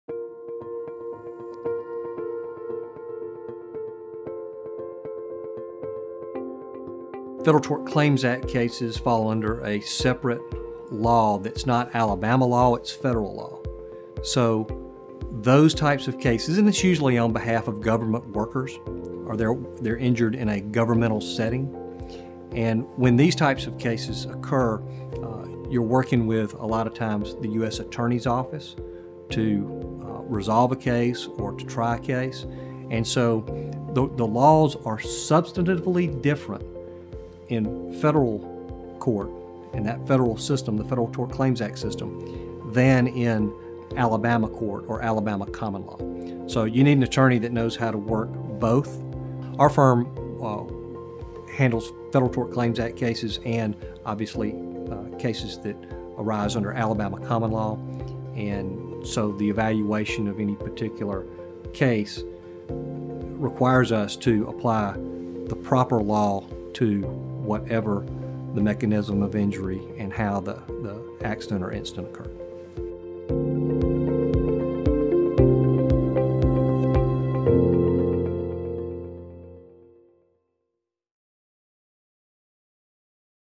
What is unique about a federal tort claims act lawsuit?
Attorney Ken Riley explains what happens when a federal torts claim act lawsuit occurs and why it is important for your attorney to know federal and Alabama common law.

Farris, Riley & Pitt, LLP
505 20th St N Ste 1700